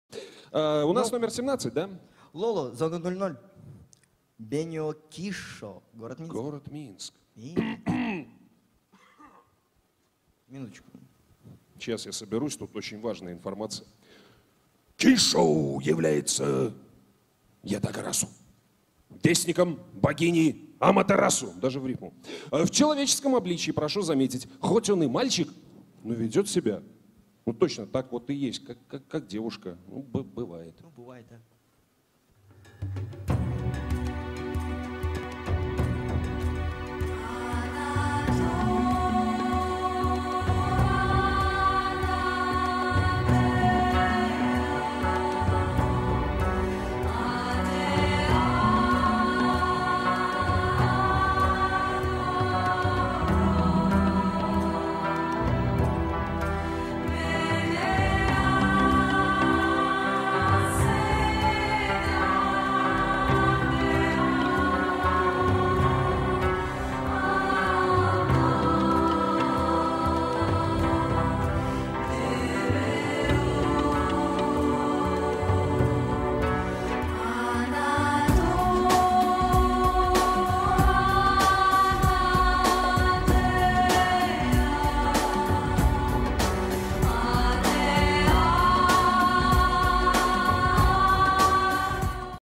FTF-2015 - Одиночное дефиле, №17 (Zone 00)
FreeTime-Fest 2015 - Belarus, Gomel

Одиночное дефиле, №17
Lolo, г.Минск
Zone 00 (Benio / Kisshou)

Огромное спасибо Владимиру Вихареву за съемку и монтаж, и Александру Шилову - за съемку на 2ю камеру!